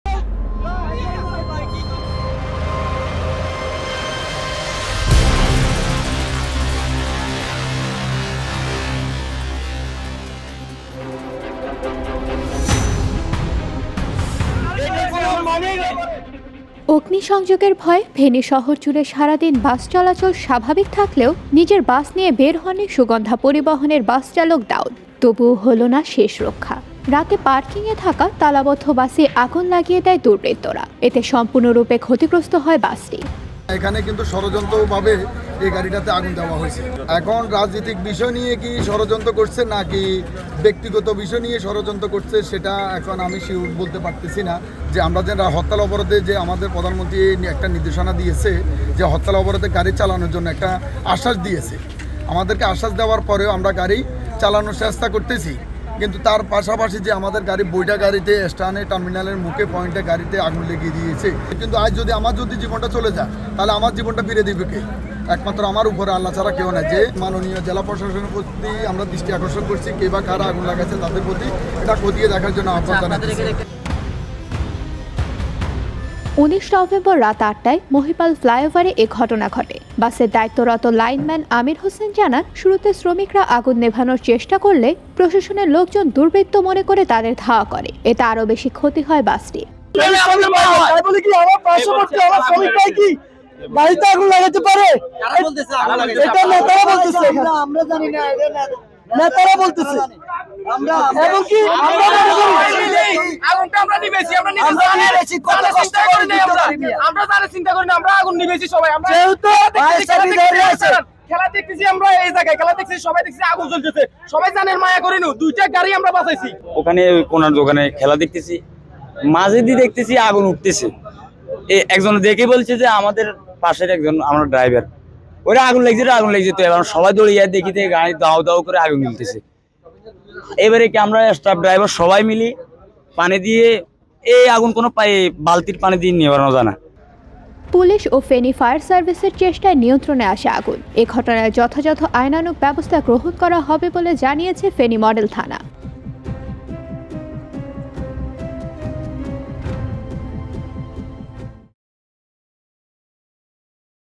অ'গ্নিকা'ণ্ডের ভয়ে বের করেননি বাস; তালাবদ্ধ বাসেই আ'গু'ন!
অগ্নিসংযোগের ভয়ে, ফেনী শহরজুড়ে সারাদিন বাস চলাচল স্বাভাবিক থাকলেও, নিজের বাস নিয়ে বের হননী সুগন্ধা পরিবহনের বাস চালক দাউদ। তবুও হল না শেষ রক্ষা! রাতে পারকিং এ থাকা তালাবদ্ধ বাসেই আগুন লাগিয়ে দেয় দুর্বৃত্তরা। এতে সম্পূর্ণ রূপে ক্ষতিগ্রস্থ হয় বাসটি।